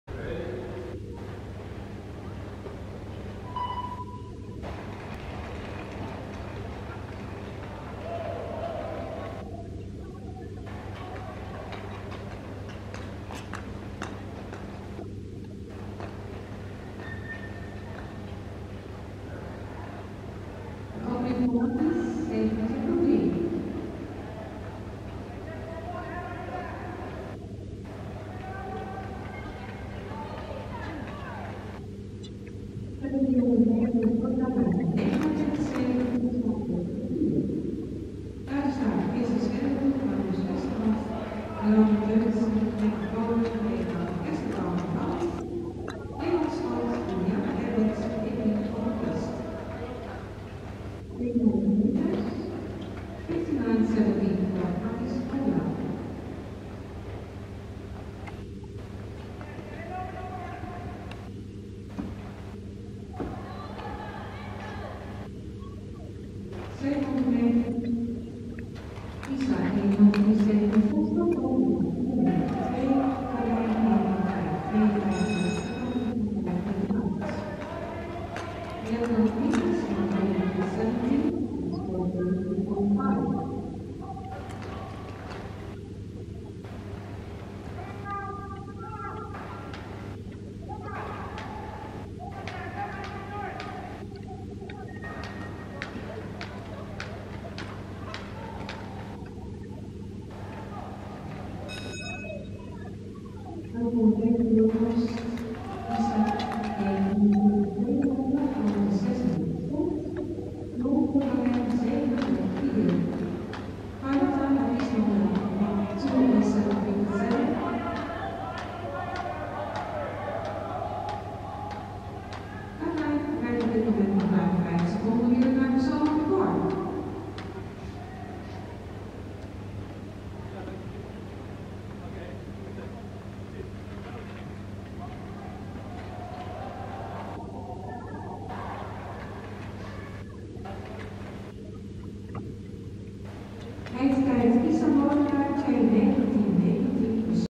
Isa Molenaar vs Carlijn Kaaij 1500 Thialf 20191027
Isa Molenaar vs Carlijn Kaaij
1500 meter
27 oktober 2019, Thialf Heerenveen
Isa 2.19.19, Carlijn 2.21.31